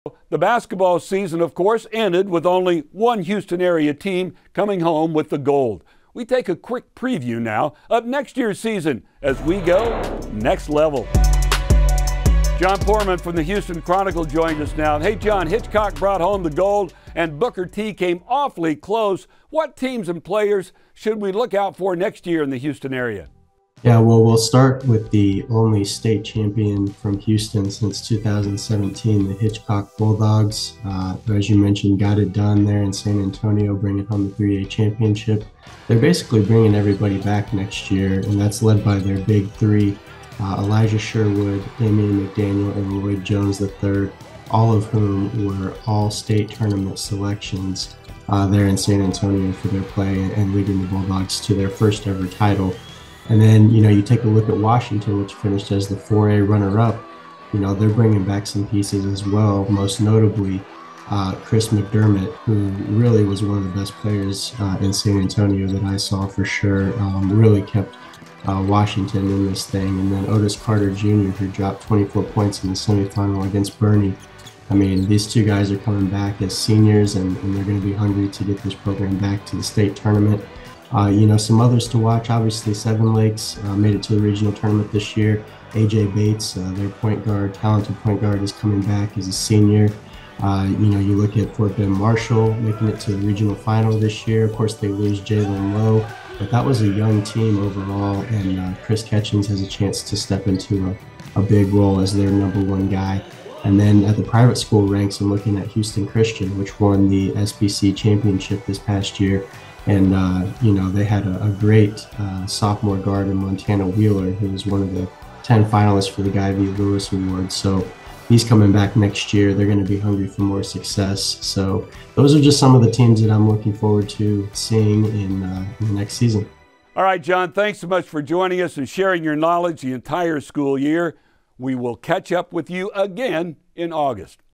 Hoops '23 Preview
On this week's  Next Level, Houston Chronicle writer, Jon Poorman, talks about what teams and players to look out for in the 2023 Hoops Season